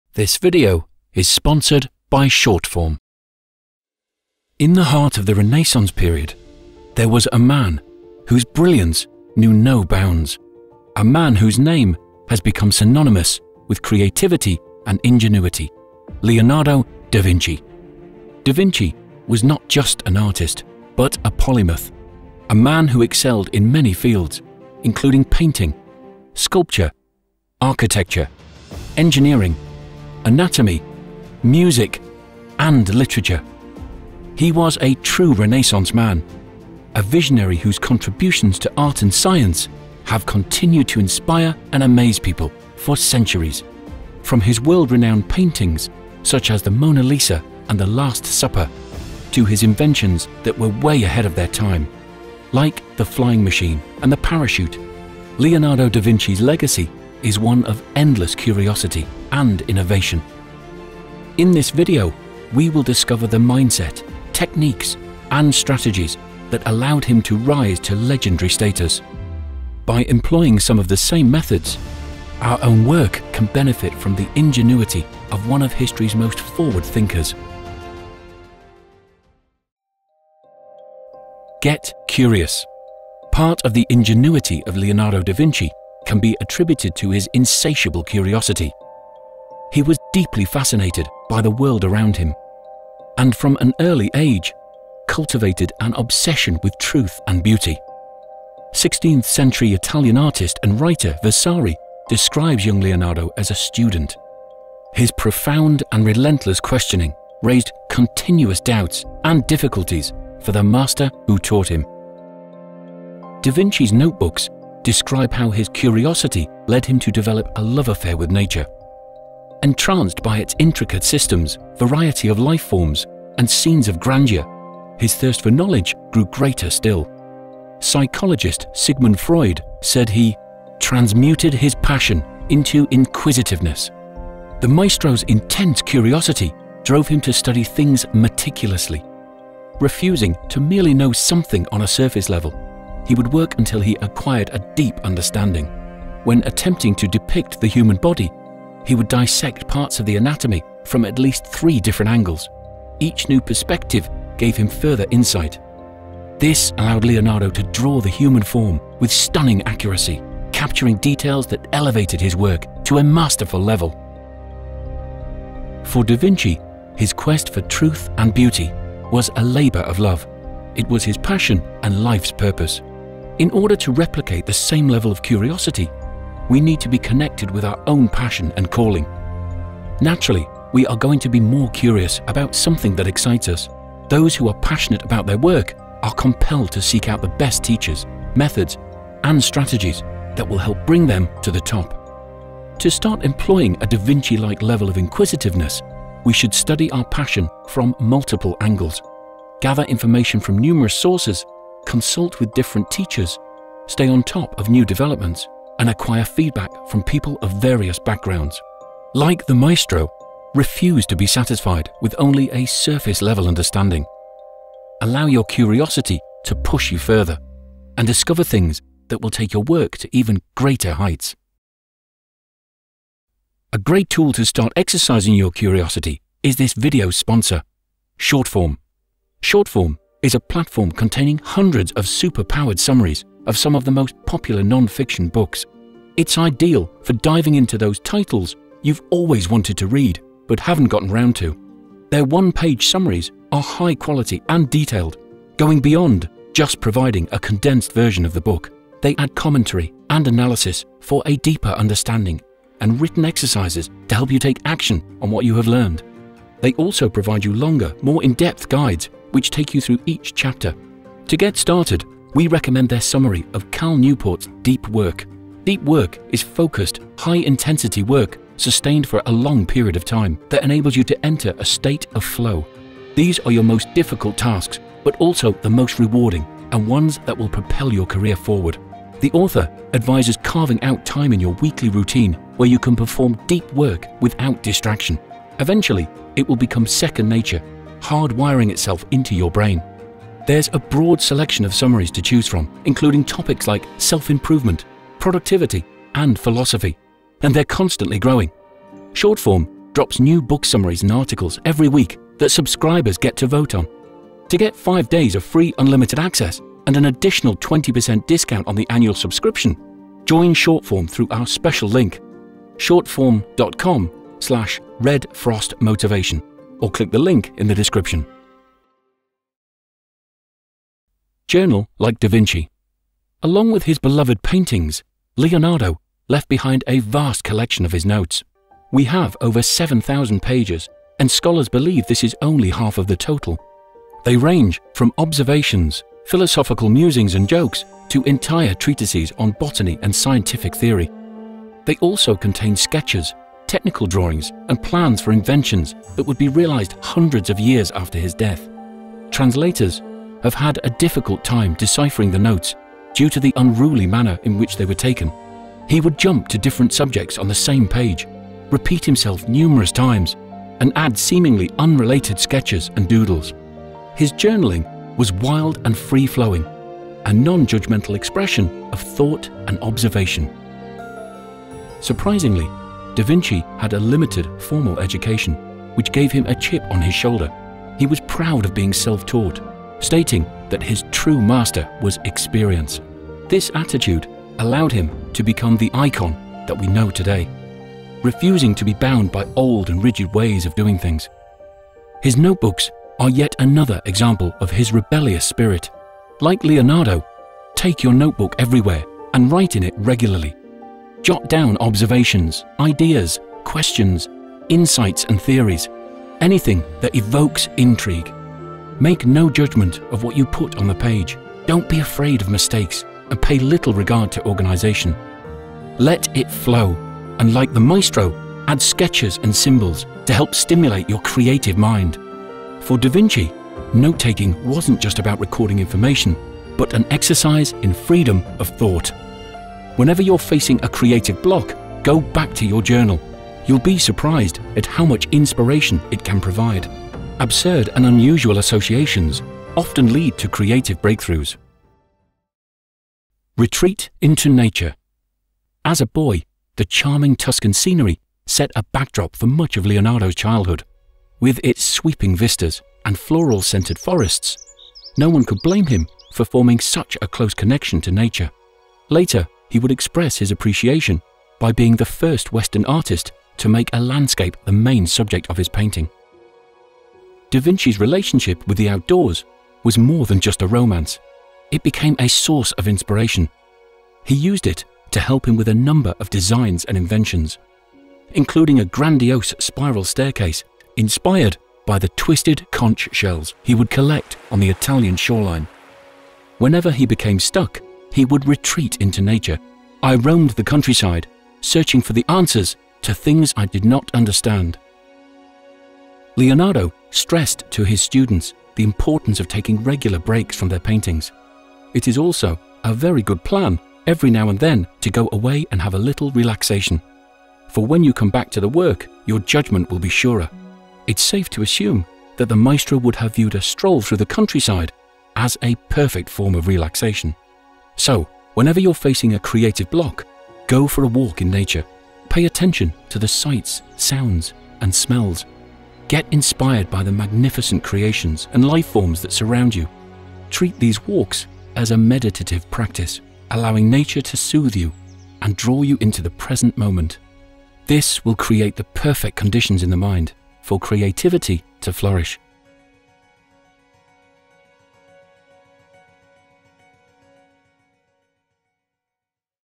Work Like da Vinci: 3 insights to awaken your genius
Leonardo da Vinci, a man who mastered many fields and left behind a legacy of innovation and beauty. In this video discover 3 insights from the maestro that enabled him to produce work on an iconic level. By applying some of the same principles, our own work can benefit from the ingenuity of one of histories most forward thinkers.

Our channel is not monetized by YouTube so this helps us to continue bringing you content.